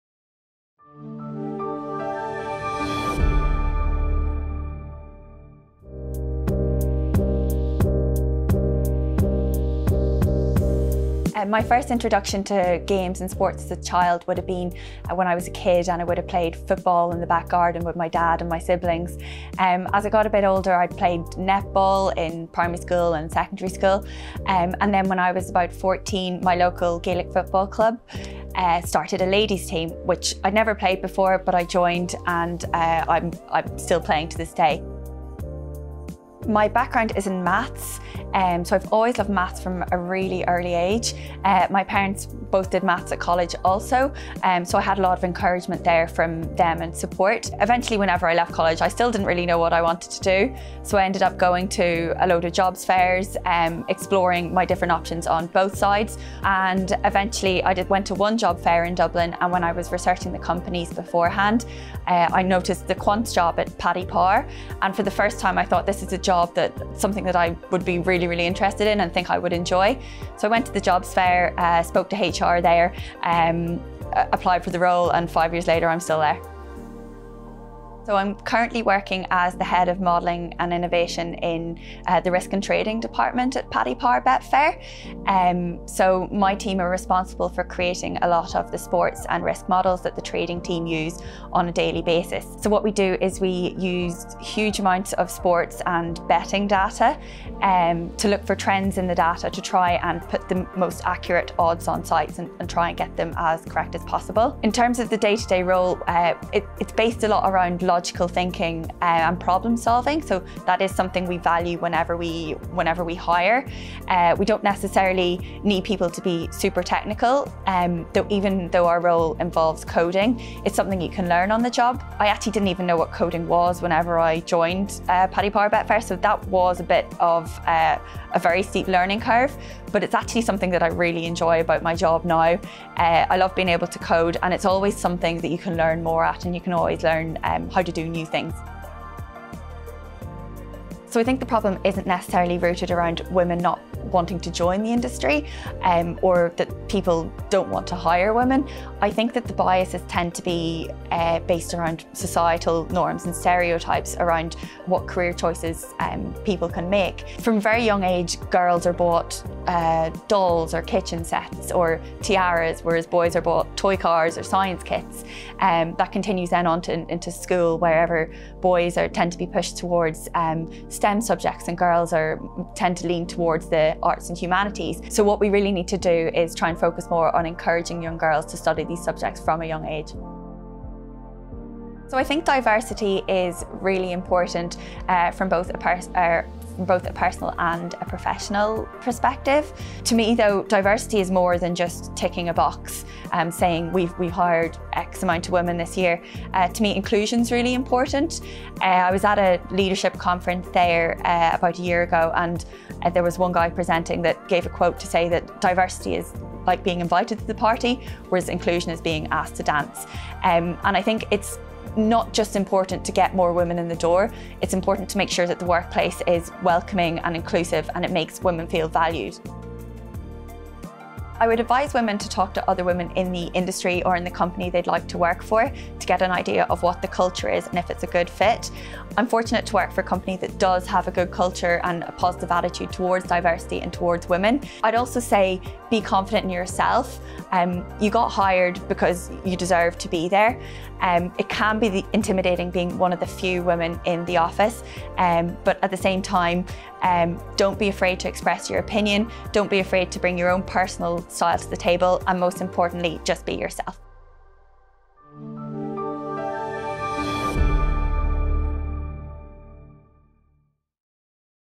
Women in Gaming Stories: Niamh Daly
Learn how Niamh Daly (Paddy Power Betfair) turned her passion for Mathematics into an exciting career in iGaming industry. Hear her thoughts on the importance of STEM education and having inclusive workplaces for women.

About Women in Gaming
Our mission is to champion diversity in games, encourage more women into leadership positions and work together to inspire change.